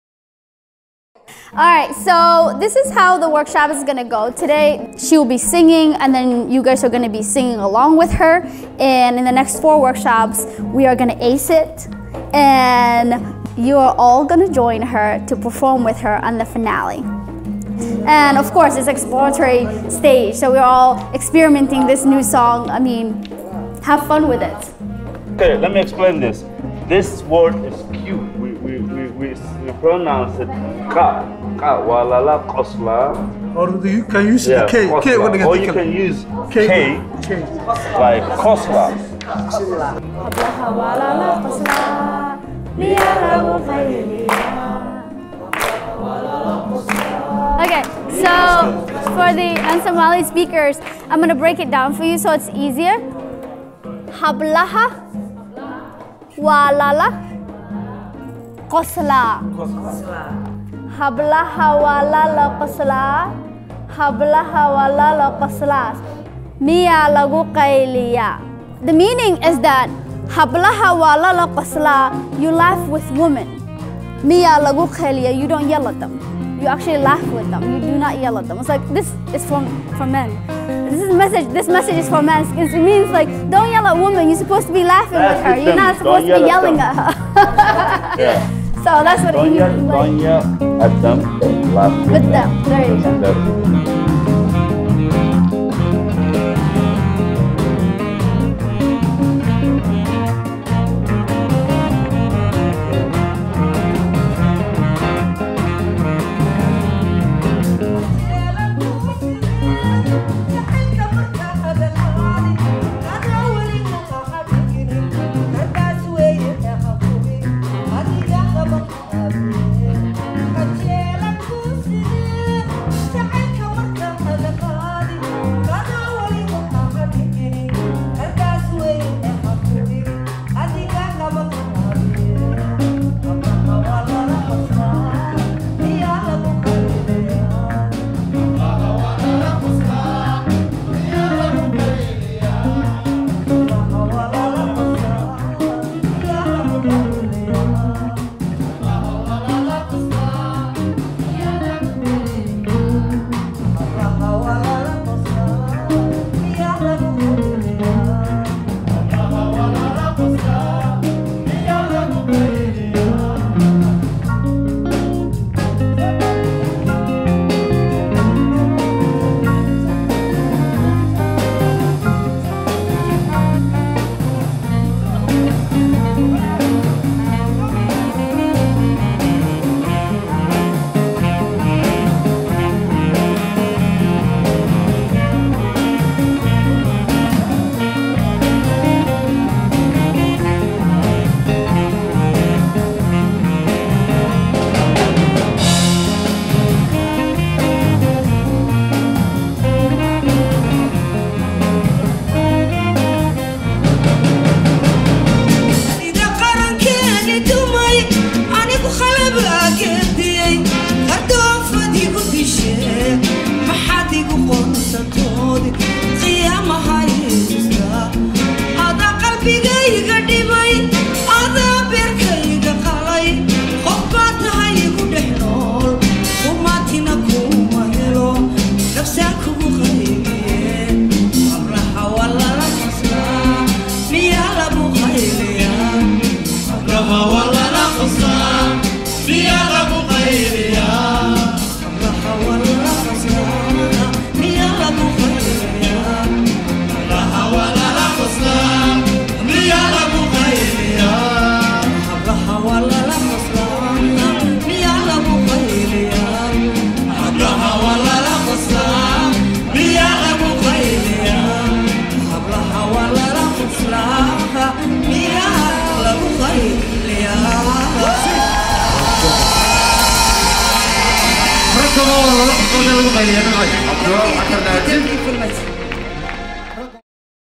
Singing along with Nimco Yasin
London-based Somali singer Nimco Yasin’s passion is teaching. Each summer, she works with young people in Jigjiga, the Somali region of Ethiopia, to mentor them in traditional song, dance, and performance.

During her residency at The Cedar, she took her turn at mentoring local Somali artists and students from Augsburg College. From learning how to pronounce the Somali song lyrics to adding instrumentation and synchronized dance moves, check out this behind-the-scenes video of what went into preparing the group for the finale performance!